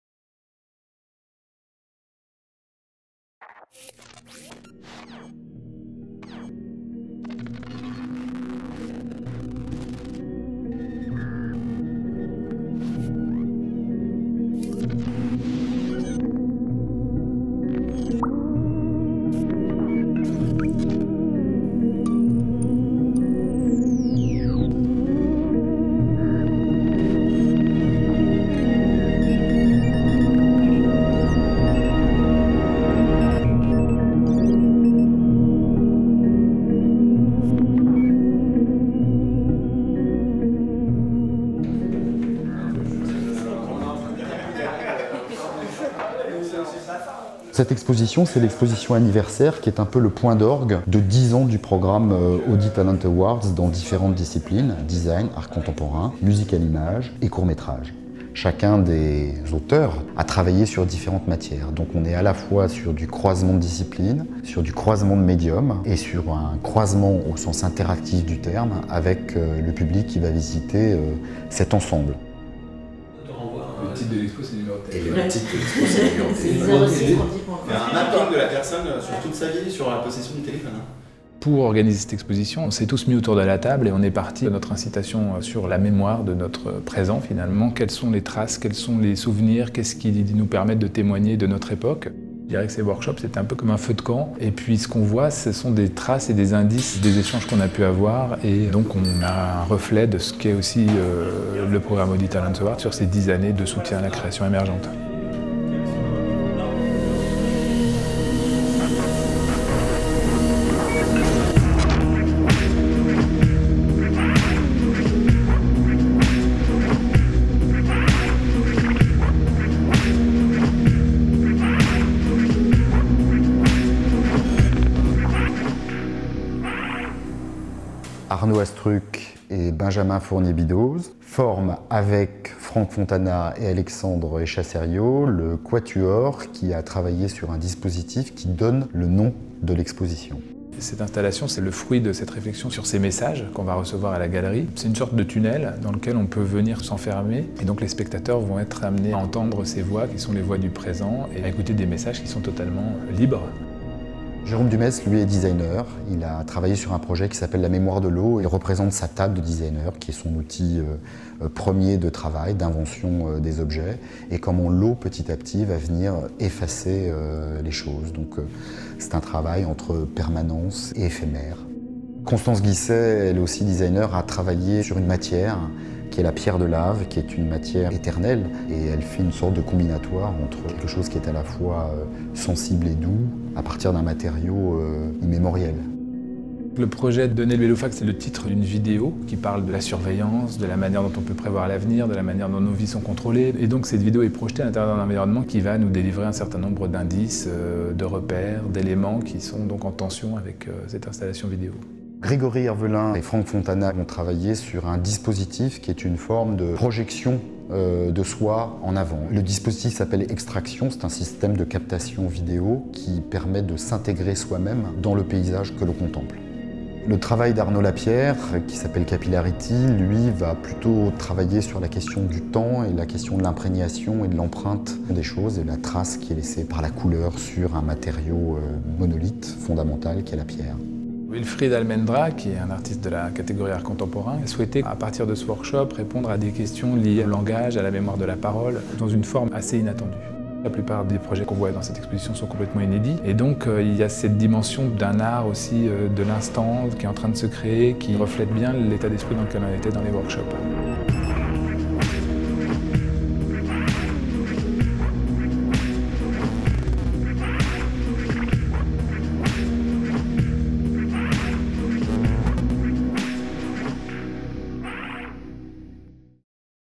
Galerie Audi talents - Visite guidée de l'exposition collective anniversaire
Dans le cadre de l’exposition anniversaire des 10 ans Audi talents awards, la Galerie Audi talents présente l’exposition interdisciplinaire collective, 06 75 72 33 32, Live a message.
Réalisée par 10 lauréats Audi talents awards : Wilfrid Almendra, Arnaud Astruc & Benjamin Fournier-Bidoz, Neïl Beloufa, Jérôme Dumetz, Alexandre Echasseriau, Franck Fontana, Constance Guisset, Gregory Hervelin, Thomas Karagiannis et Arnaud Lapierre.
Une proposition de Vincent Carry, Gaël Charbau, René-Jacques Mayer, Carole Scotta.